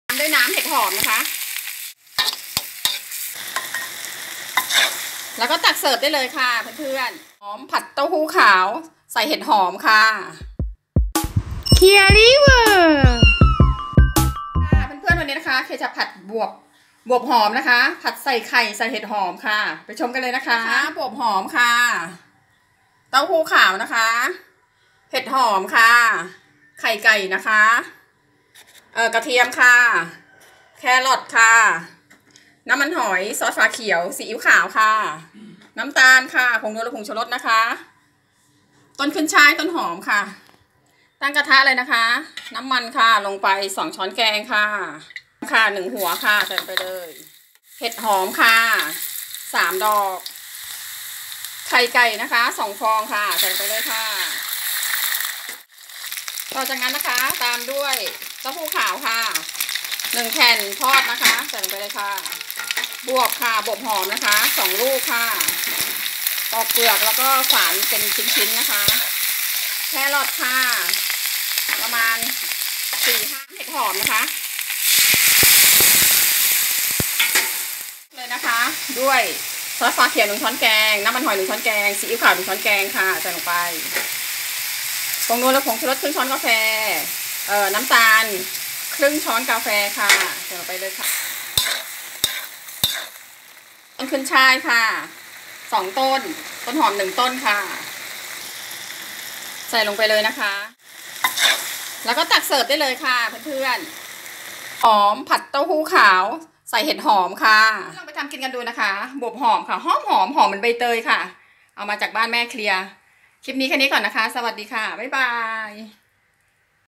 ผัดบวบหอมเต้าหู้ขาว
ผัดบวบหอมเต้าหู้ขาวใส่ไข่เห็ดหอม
1.บวบหอม/2ลูก
2.ไข่/2ฟอง
3.เห็ดหอม/3ดอก
4.เต้าหู้ขาว/1แผ่น
5.ต้นขึ้นช่าย/2ต้น
6.ต้นหอม/1ต้น
7.แครอท/5ช้อน
8.ซอสฝาเขียว/1ช้อนแกง
9.ซีอื้วขาว/1ช้อนแกง
10.น้ำมันหอย/1ช้อนแกง
11.ผงชูรสครึ่งช้อนกาแฟ
12.น้ำตาลครึ่งช้อนกาแฟ